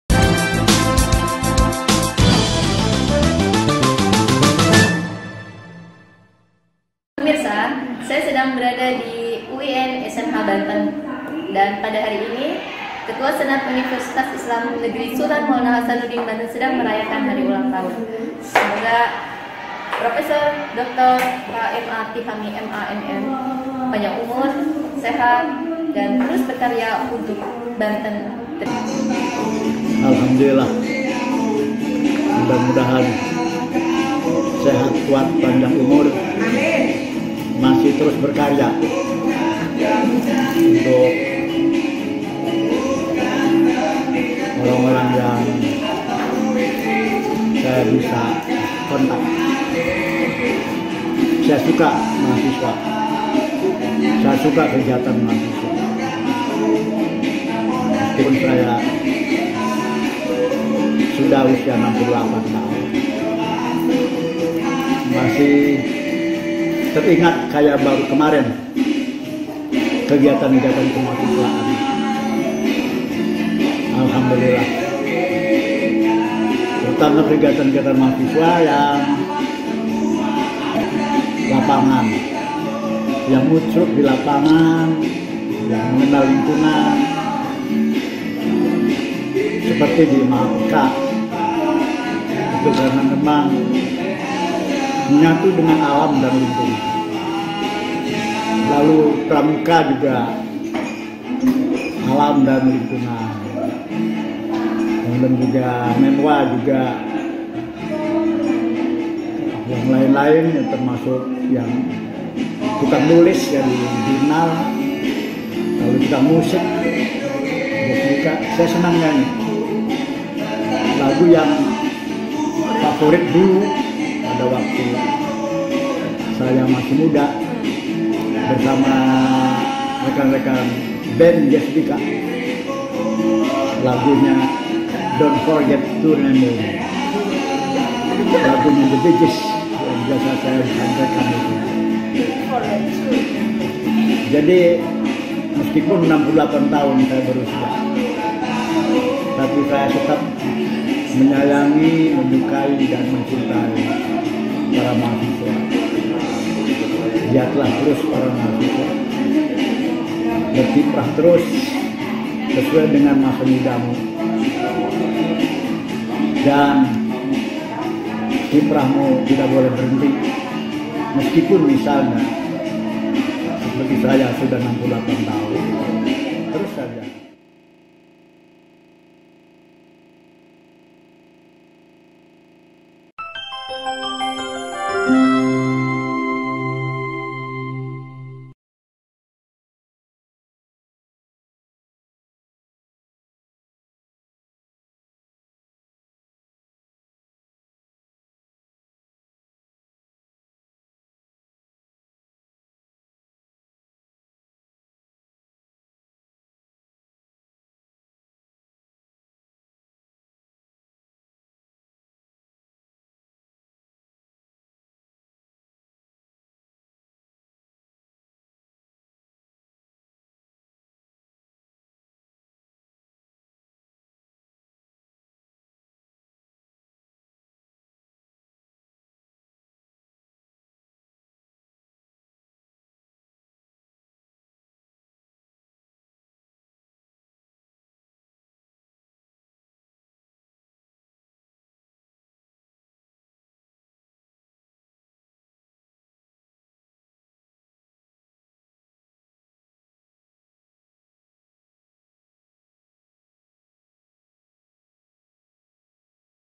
Milad ke 68 Guru Besar UIN SMH Banten
Milad ke 68 Guru Besar dan Ketua Senat Universitas sebagai Anggota Kehormatan MAHAPEKA UIN SMH Banten  dan Tokoh Banten Barakahallah Fi Umrik. Amin Ya Robal 'Alamin.